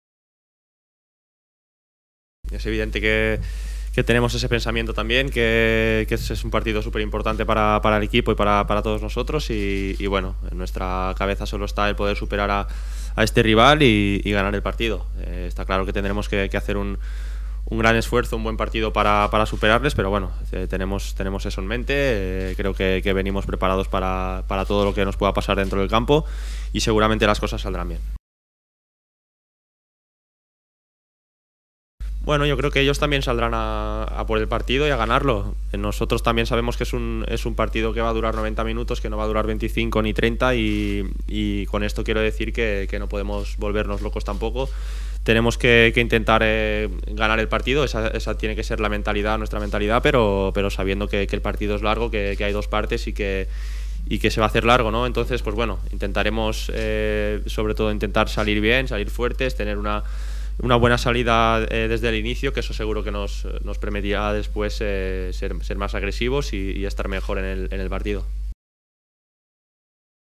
Rueda de prensa de Bruno Soriano en la previa del FC Zürich - Villarreal CF
Rueda de prensa de Bruno Soriano en la previa del FC Zürich - Villarreal CF de Europa League